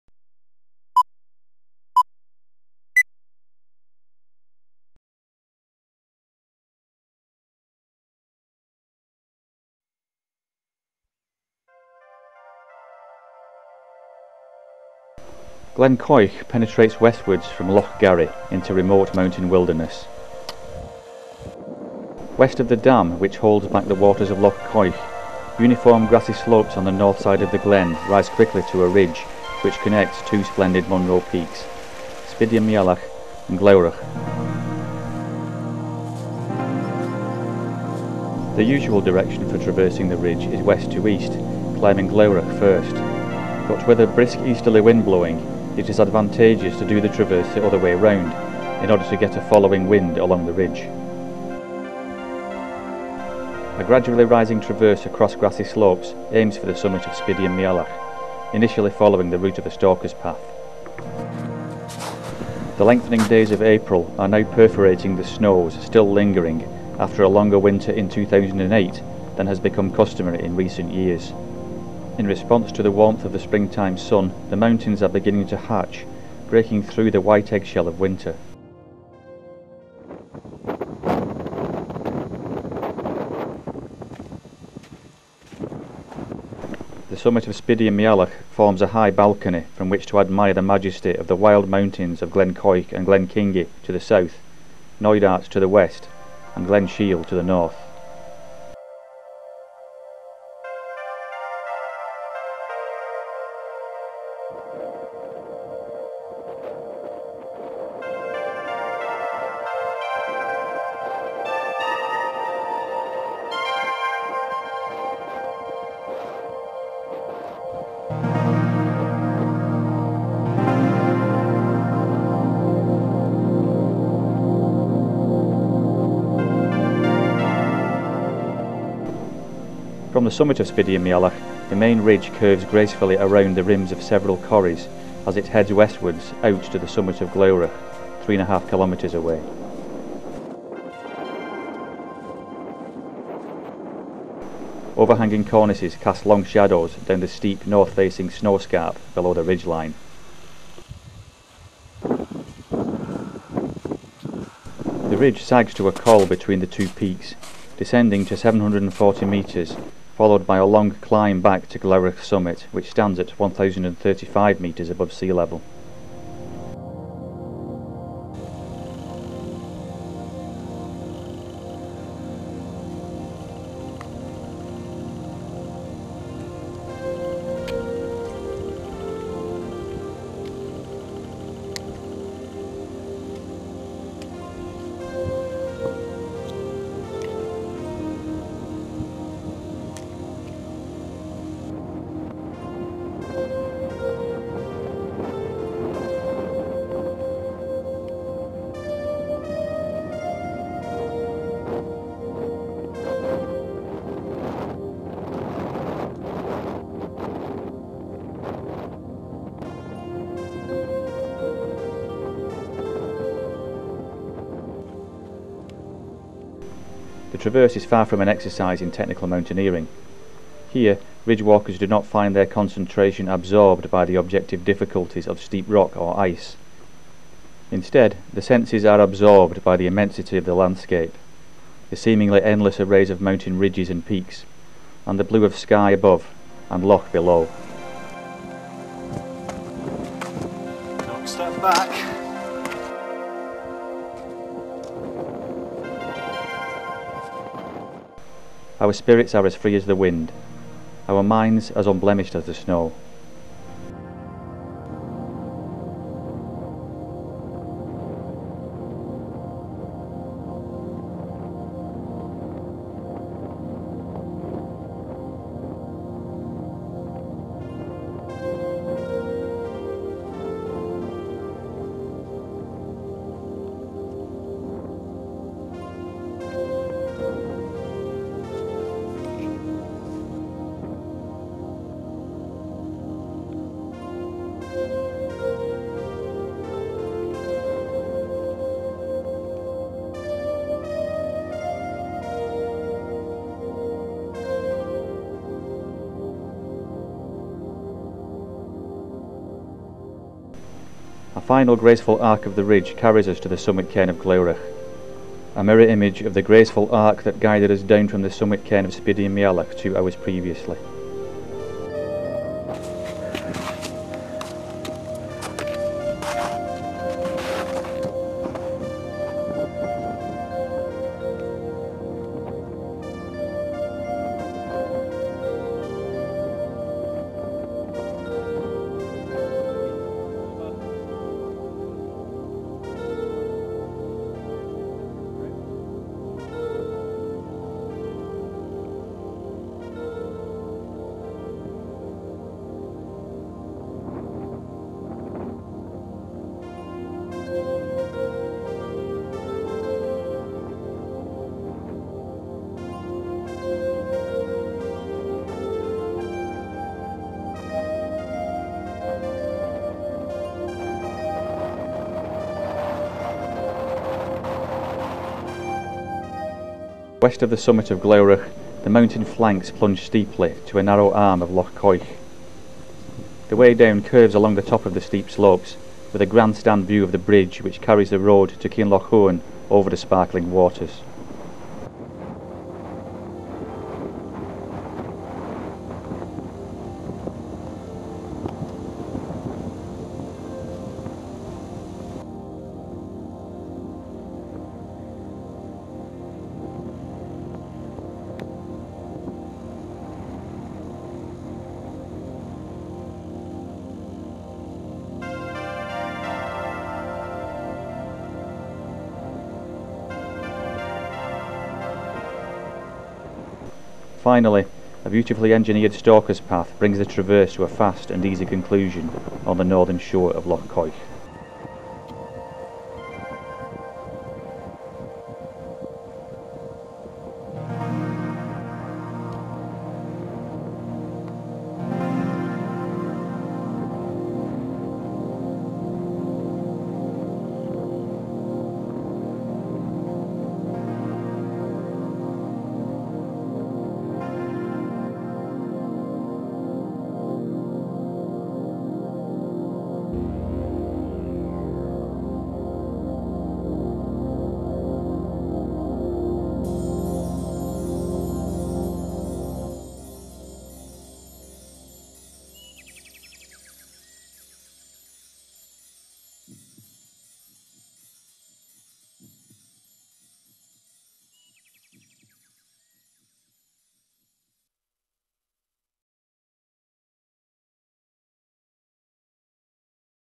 Glen Quoich High Way
a mountain ridge walk in the West Highlands of Scotland.